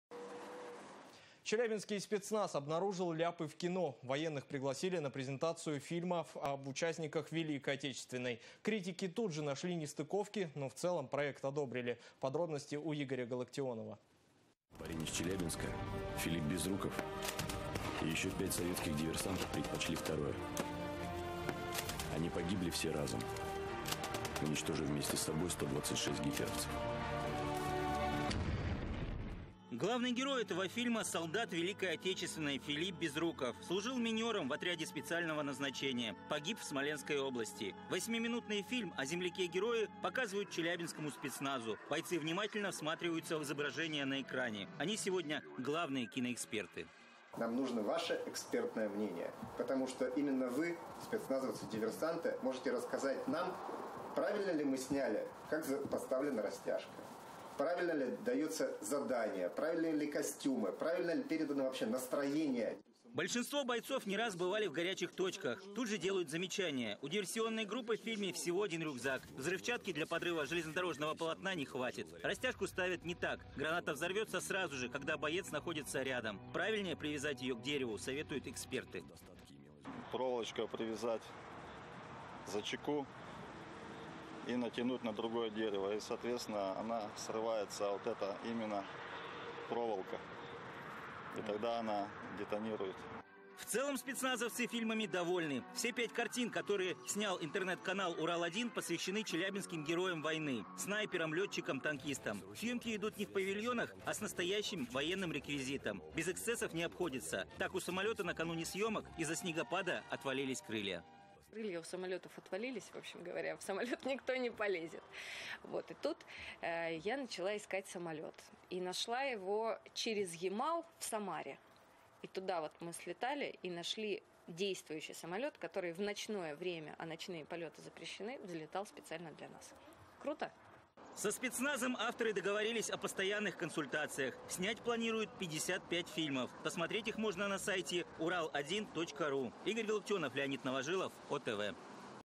Челябинские спецназовцы провели экспертизу фильмов из цикла "Город героев"
Челябинский спецназ обнаружил ляпы в кино! Военных пригласили на презентацию фильмов об участниках Великой Отечественной. Критики тут же нашли нестыковки, но в целом проект одобрили. Подробности — у Игоря Галактионова.